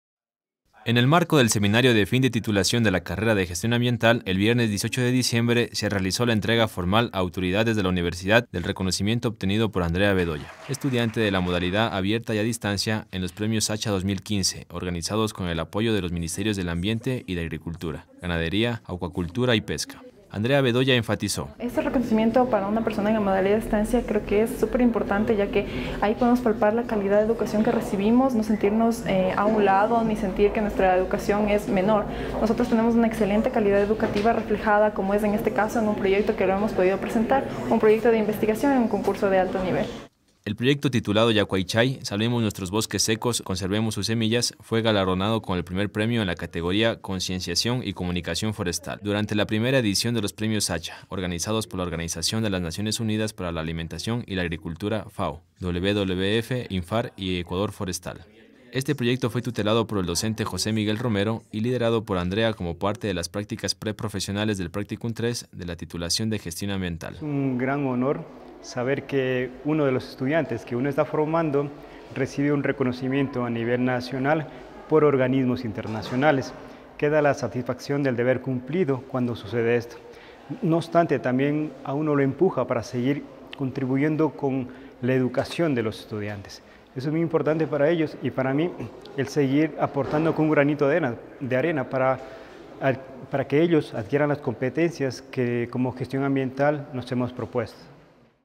Estudiante de la modalidad a distancia realizó entrega de Premio Sacha a autoridades
Andrea Bedoya, estudiante de Gestión Ambiental de la modalidad a distancia y ganadora del Premio Sachai 2015, efectuó la entrega de dicho reconocimiento a las autoridades de la Universidad.